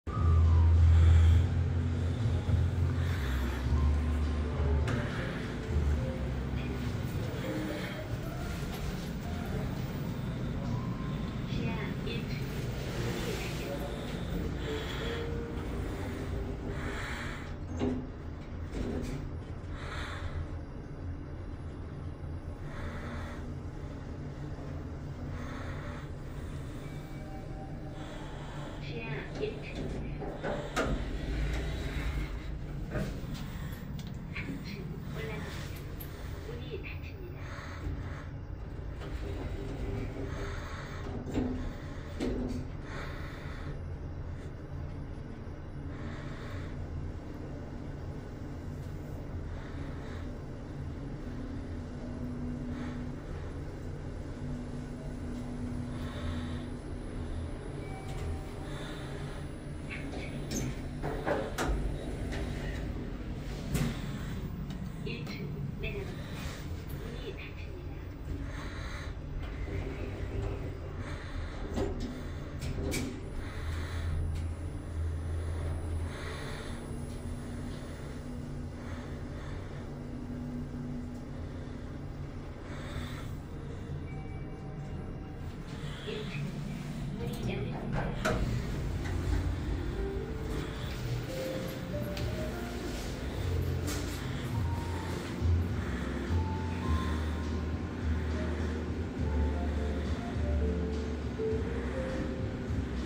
광주광역시 ACC디자인호텔 정문쪽 B3,B1~3층 운행용 티센크루프엘리베이터(티케이엘리베이터) 탑사기(2023.05.22 촬영)
건물 안쪽 메인구간 B3~12층 운행용 왼쪽호기/B2~12층 운행용 오른쪽호기(1000kg 15인승, 오른쪽호기랑 붙어있는 외부운전반을 누르면 2대연동운행하고
왼쪽호기랑 붙어있는 외부운전반을 누르면 왼쪽호기만 단독호출되는 것으로 추정됩니다.)랑
정문쪽 B3,B1~3층 운행용 1대짜리(750kg 11인승)를 운용중인데
전부 블랙 스테인레스스틸 사양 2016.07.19 Evolution1(90m/min)입니다.

B1층은 지하주차장, 1층은 로비/프론트데스크/비즈니스센터/카페에르모소(HERMOSO), 2층은 한정식&일식당/연회장, 3층은 객실/소회의실, 4~10층은 객실, 11층은 TENONE 라운지바가 있으며,
출입통제구역인 B3~B2층 기계설비실/창고랑 12층 옥상은 직원용 카드키가 필요합니다.

2016 Thyssenkrupp Evolution1 MRL Traction Elevator at ACC Design Hotel, Gwangju Metropolitan City, South Korea.